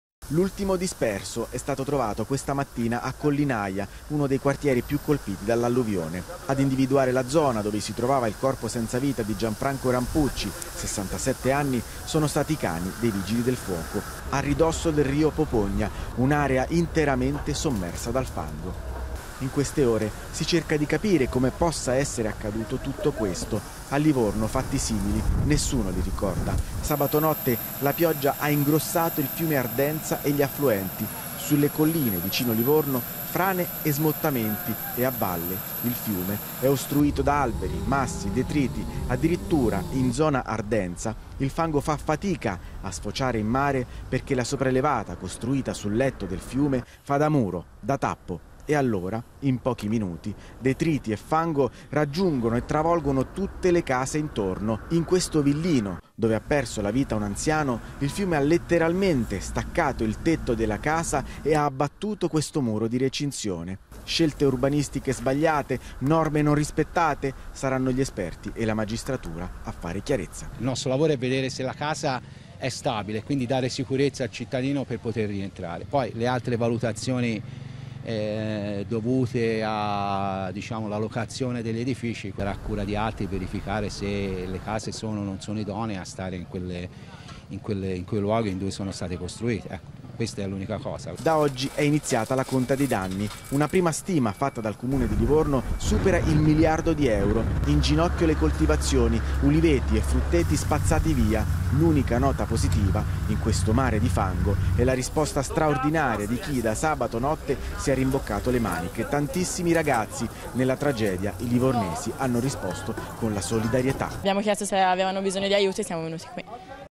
Livorno piange le sue 8 vittime. La vicinanza del Presidente della CEI alle famiglie in lutto
E' stato ritrovato, nel fango non lontano dalla sua abitazione il corpo dell'ultimo disperso nel nubifragio di Livorno Gianfranco Tampucci di 67 anni. Sale cosi’ a 8 il numero delle vittime. Domani primi funerali, in forma privata. Messaggio di vicinanza del presidente della Cei Gualtiero Bassetti. Grandi gli esempi di solidarieta' tra il fango. L'inviato Vito D'Ettorre